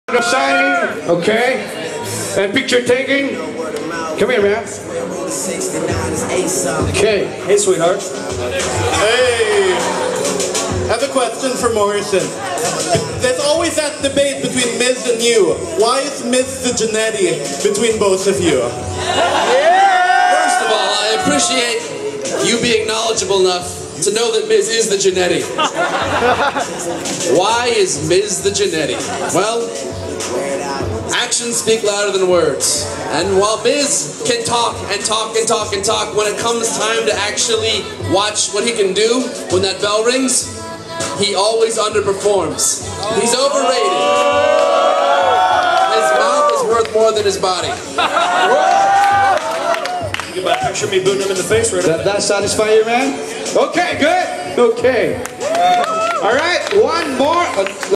John Morrison explains why The Miz is the "Jannetty" (WWFX)
John Morrison, Luke Gallows, MVP, and Melina did a meet and greet at geenbelt in the Philippines for their WWFX tour. During the Q&A section, Morrison was asked to explain what makes the Miz the "Janetty"  of their tag team.